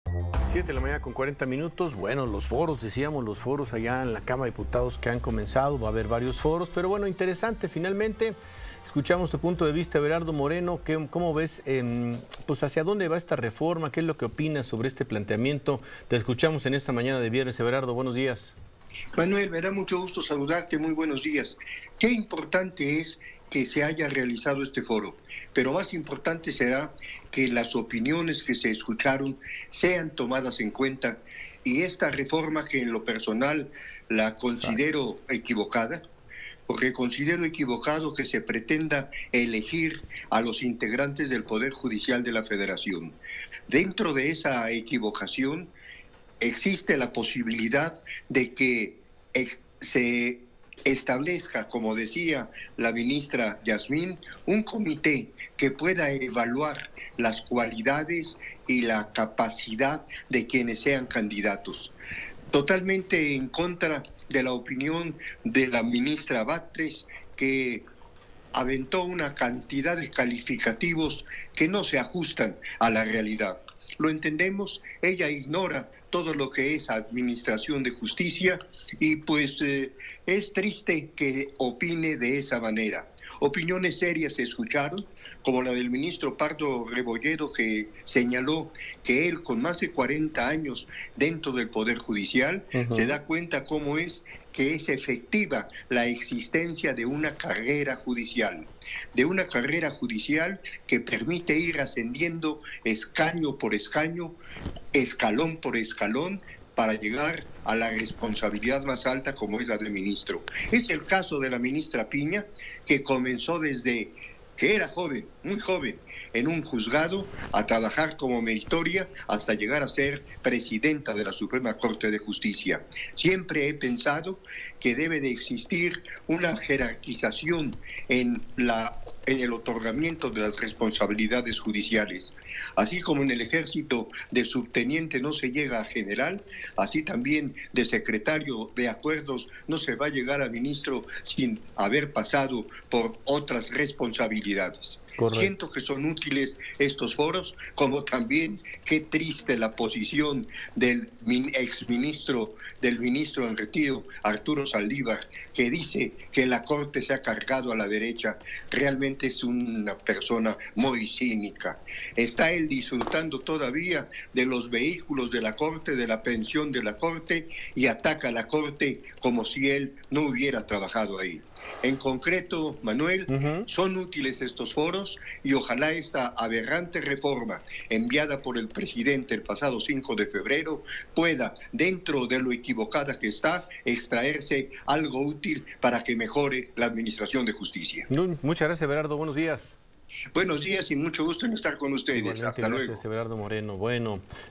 Everardo Moreno tunde a Zaldívar: es un cínico que disfruta de la pensión de la Corte
Luego de arrancar los foros para reformar el Poder Judicial, Everardo Moreno dijo estar en desacuerdo con la elección de juzgadores. Asegura que es efectiva una carrera judicial para lograr la jerarquización en el Poder. El analista celebra los foros para escuchar los distintos puntos de vista y criticó la posición de Arturo Zaldívar a quien considera un cínico por criticar a la Corte mientras disfruta de su pensión vitalicia.

Grupo Fórmula